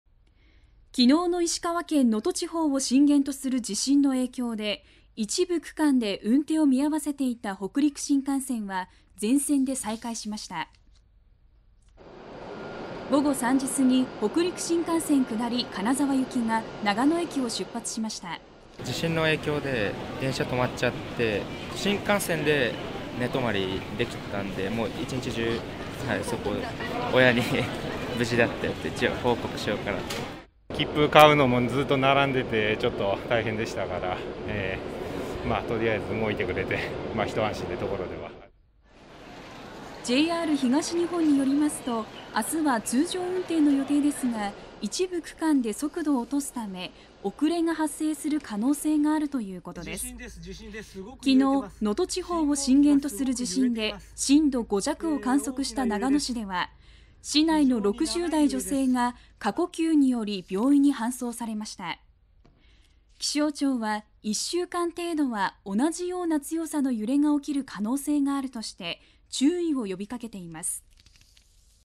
「新幹線で寝泊りした」地震で運転見合わせの北陸新幹線が全線再開（abnニュース 2024.01.02）
1日の石川県能登地方を震源とする地震の影響で一部区間で運転を見合わせていた北陸新幹線は全線で再開しました。

午後3時すぎ、北陸新幹線下り・金沢行きが長野駅を出発しました。

■東京から富山に帰省する人「地震の影響で電車止まっちゃって、新幹線で寝泊りできたので1日中そこにいた。親に無事だと報告したい」

■金沢市へいく人「切符買うのもずっと並んでいて大変でしたから、とりあえず動いてくれてひと安心」

JR東日本によりますと、3日は通常運転の予定ですが一部区間で速度を落とすため遅れが発生する可能性があるということです。
1日、能登地方を震源とする地震で震度5弱を観測した長野市では、市内の60代女性が過呼吸により病院に搬送されました。

気象庁は、1週間程度は同じような強さの揺れが起きる可能性があるとして注意を呼び掛けています。